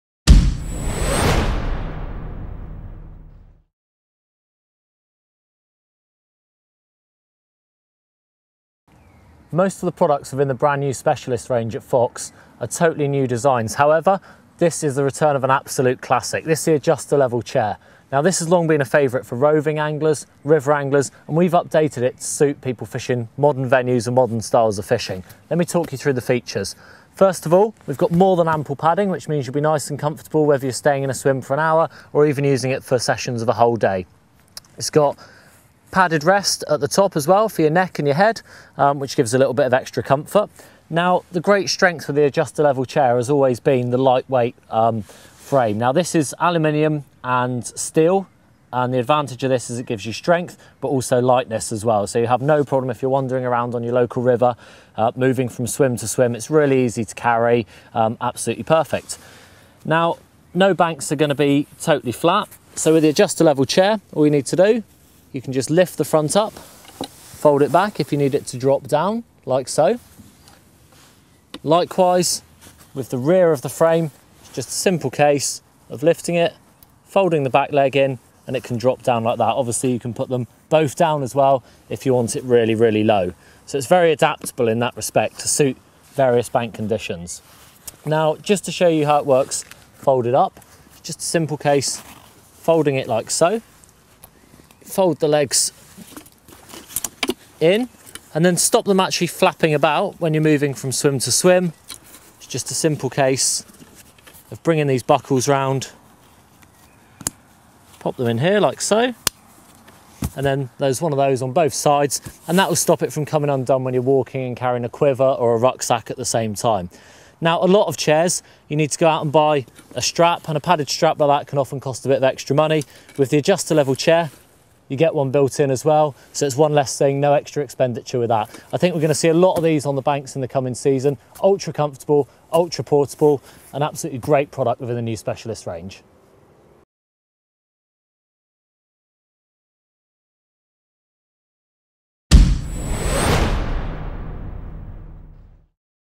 : Fox Specialist Fishing TV : Adjusta Level Chair
Back by popular demand we are resurrecting one of the most successful chairs of all time! Here is our new and improved version with Fox's Specialist consultant Julian Chidgey running you through all you need to know.